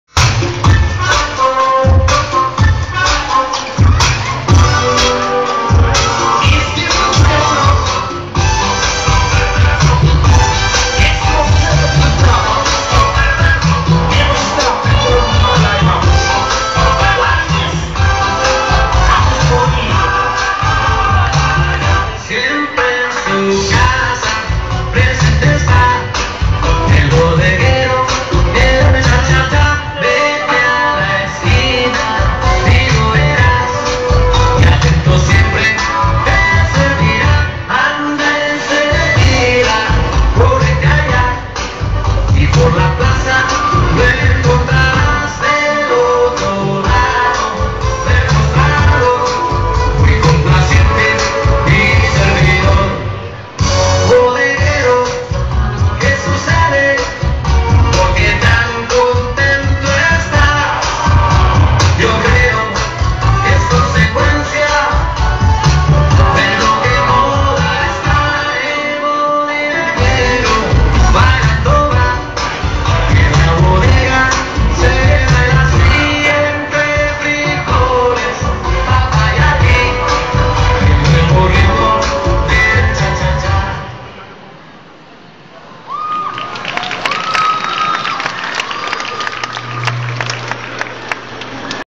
invictus (baguio) dance ..studio one show (dancesport) cha3
studio one's show at sm atrium,, june 24.. =)